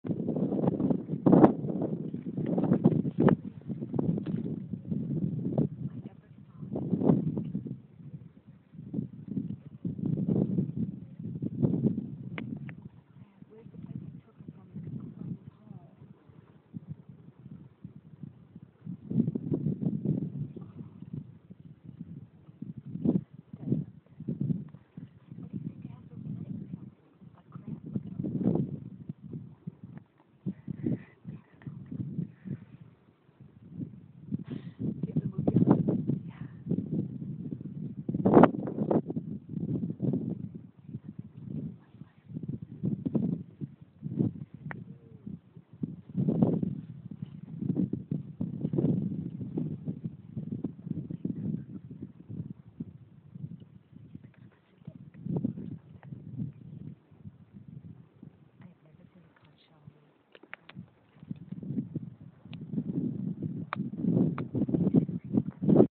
Conch shell walking away
We didn't know anything was living in it and five minutes later it starts walking away from us! how rude.